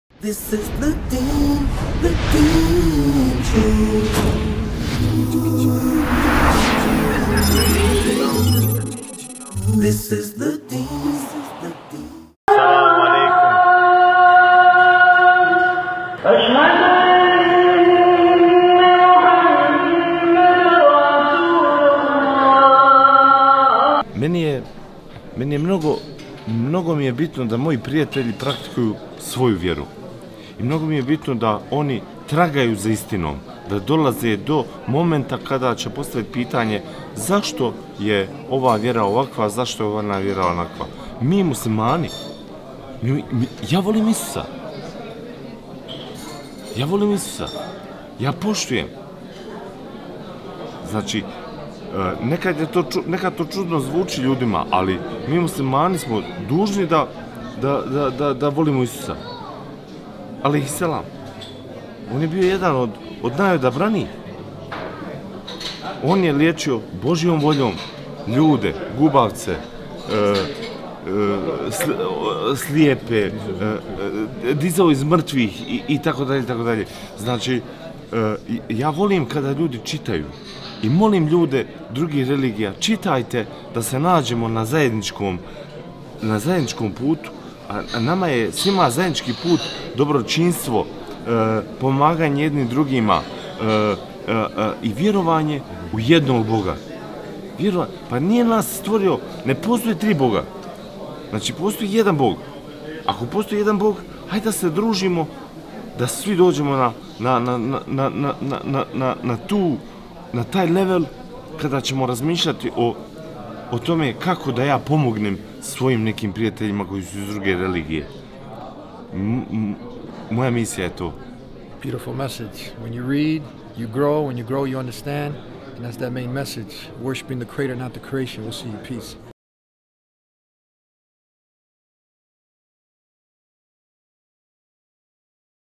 Muslims love Jesus (Muslimani vole Isusa Иисус) world champ Dževad Poturak
Muslims love Jesus (Mi muslimani volimo,
Isus, Jesus, Иисус) Dževad Poturak world champion from Bosnia